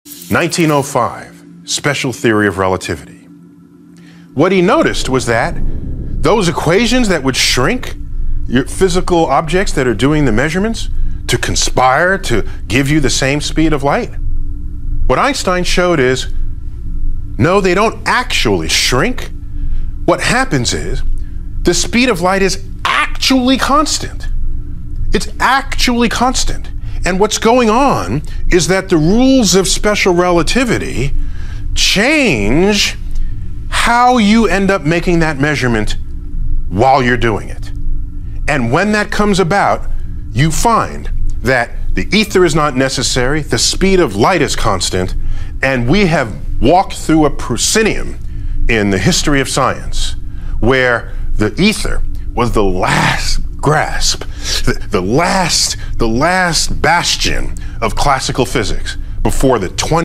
Rules of Special Relativity Change🧐 Neil deGrasse Tyson
Rules of special relativity change on how you end up making that measurement, Neil deGrasse Tyson explains. Special relativity revealed that the speed of light is a limit that can be approached but not reached by any material object. It is the origin of the most famous equation in science, E = mc2, which expresses the fact that mass and energy are the same physical entity and can be changed into each other. Related topics